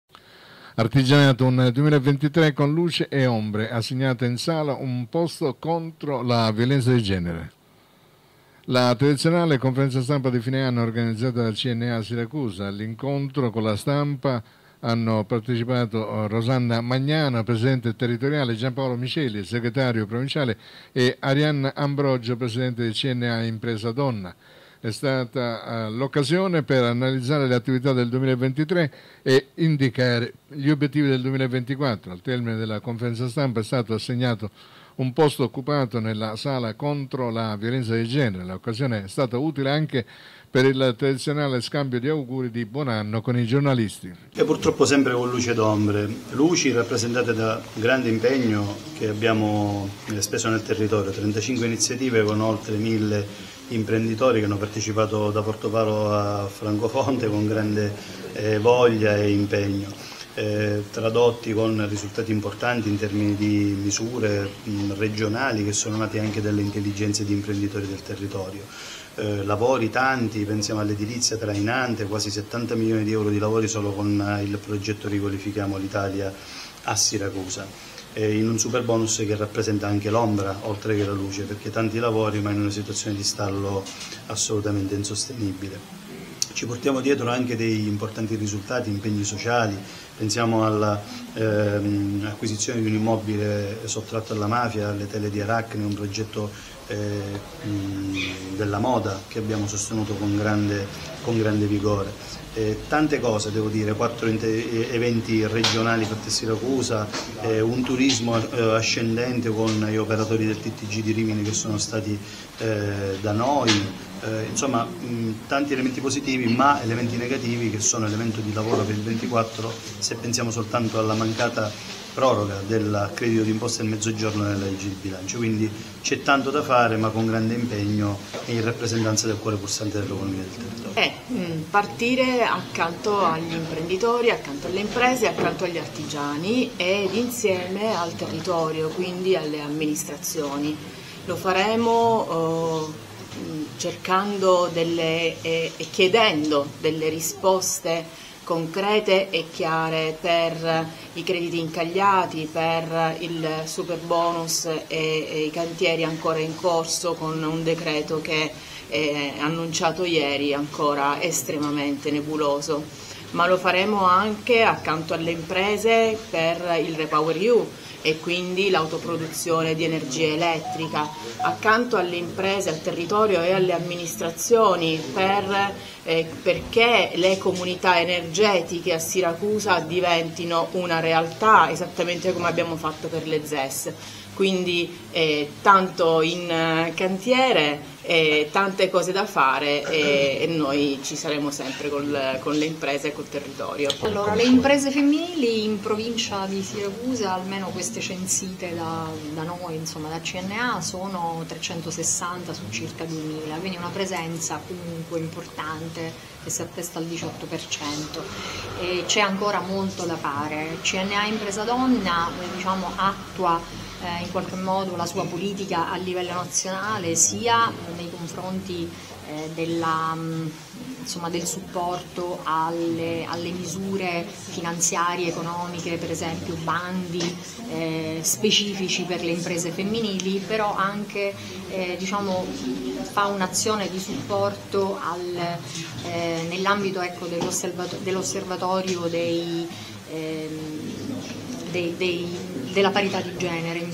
Artigianato, un 2023 con luce e ombre: assegnato in sala un posto contro la violenza di genere
La tradizionale conferenza stampa di fine anno organizzata da CNA Siracusa. All'incontro con la stampa parteciperanno Rosanna Magnano, presidente territoriale, Gianpaolo Miceli, segretario provinciale e Arianna Ambrogio, presidente di CNA Impresa Donna. E’ stata l'occasione per analizzare le attività del 2023 e indicare gli obiettivi del 2024. Al termine della conferenza stampa è stato assegnato un "posto occupato" nella sala contro la violenza di genere.
L'occasione è stata utile anche per il tradizionale scambio di auguri di buon anno con i giornalisti.